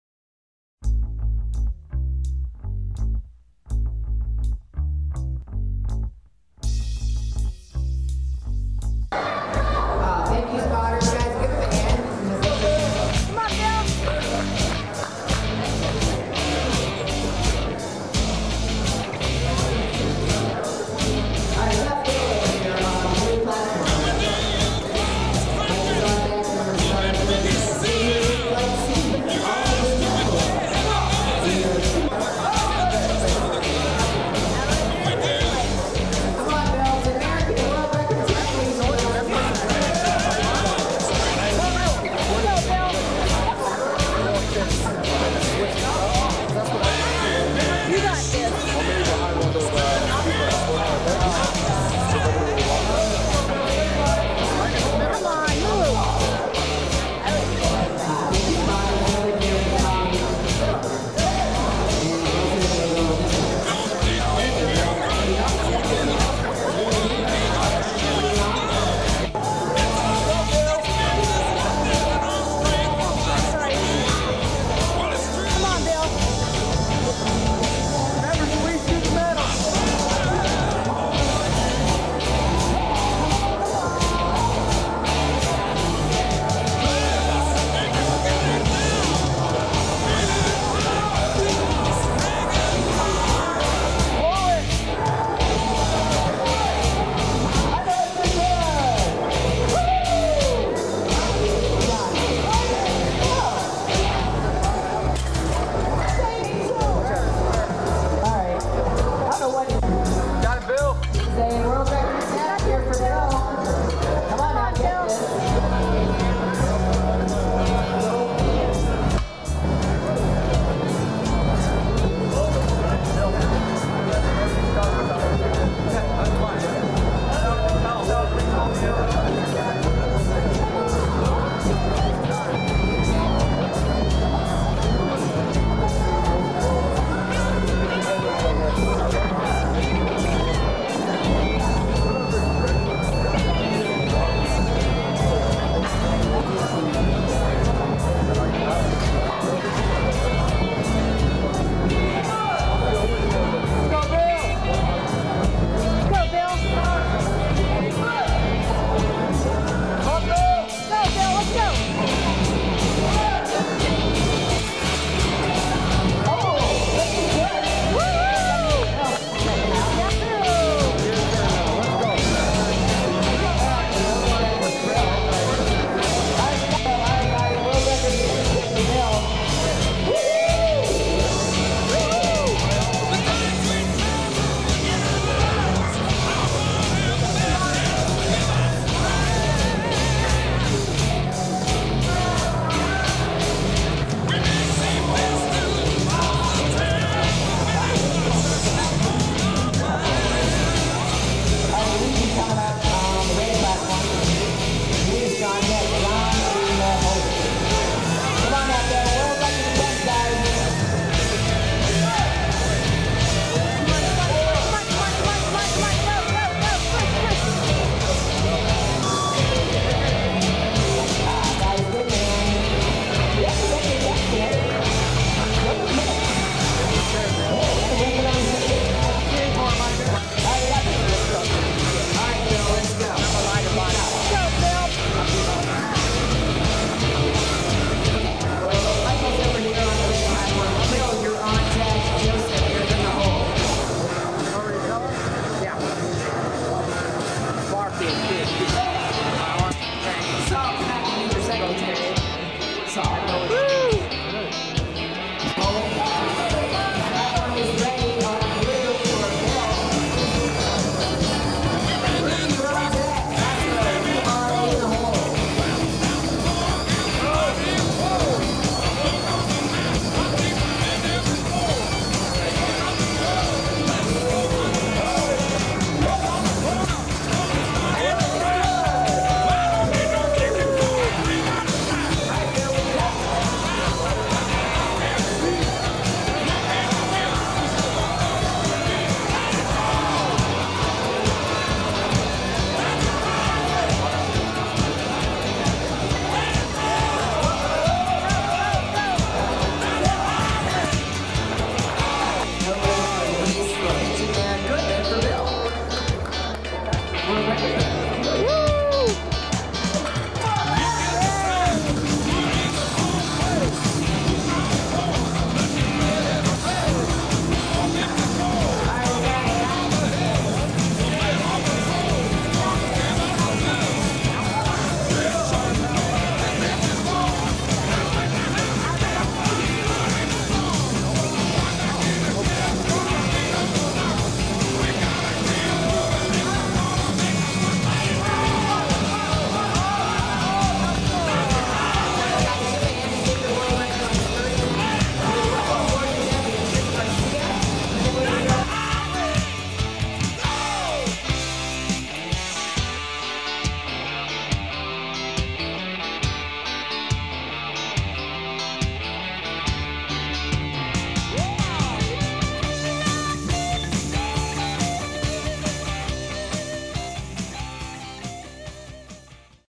Bill Carlough IPL Worlds 2013
International Powerlifting League World Championships in Las Vegas, Nevada November 7, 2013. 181# Masters Champ & Open Runner-up. Two World & American Records, 440# Bench & 1521 Total.